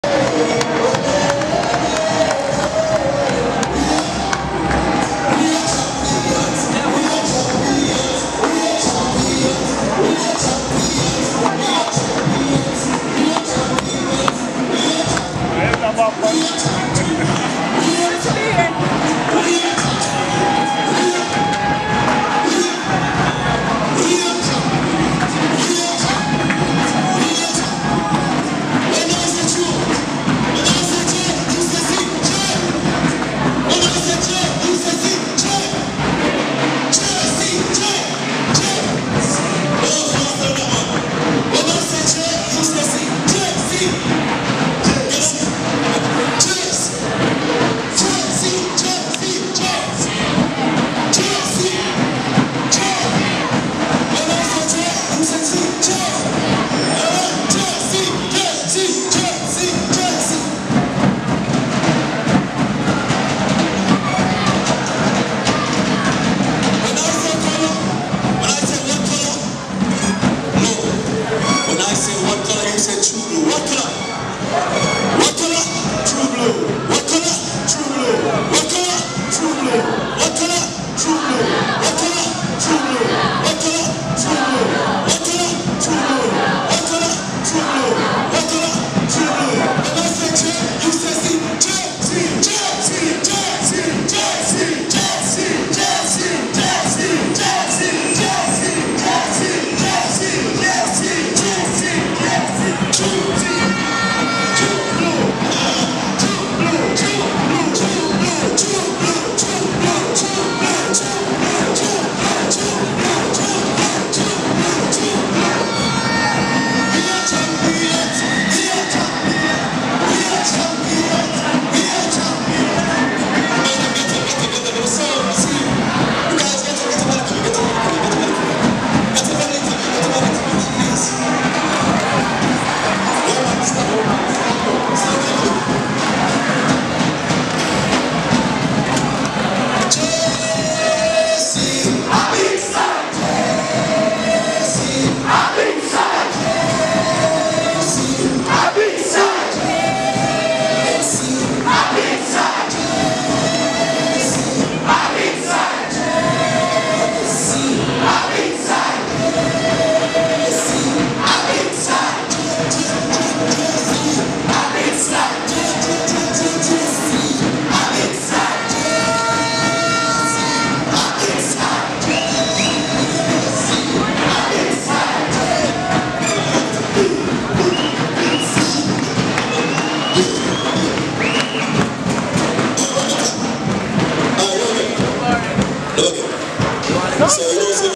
JC Celebrating with the Manning Cup Team
JC Celebrating the 31st win of the Football Manning Cup